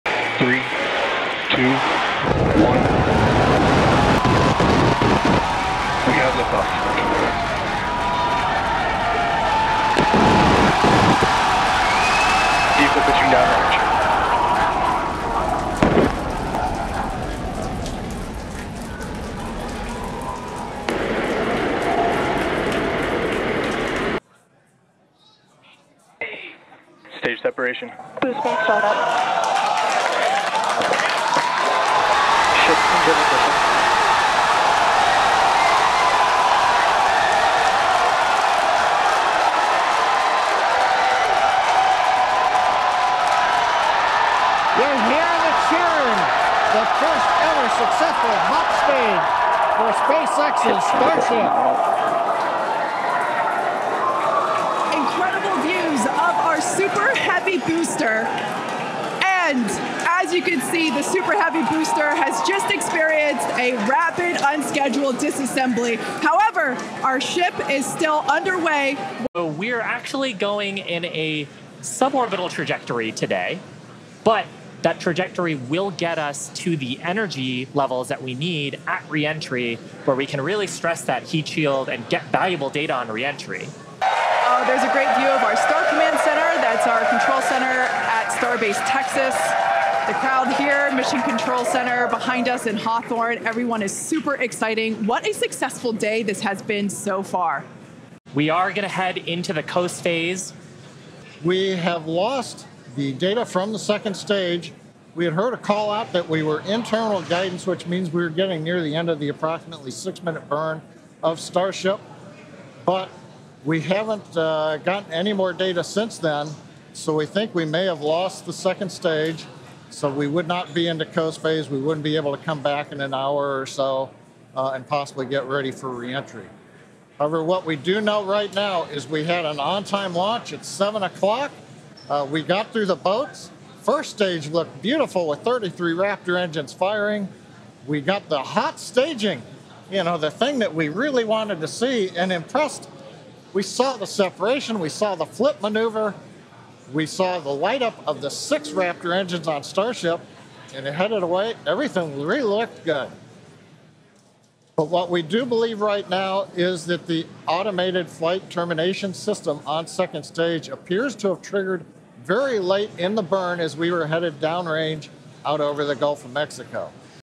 SpaceX launches second Starship test flight
Early Saturday morning, SpaceX launched a second test flight of its Starship rocket. Unlike in its the first launch seven months ago, Starship successfully separated from its booster engines but lost contact minutes later and initiated a self-destruction protocol.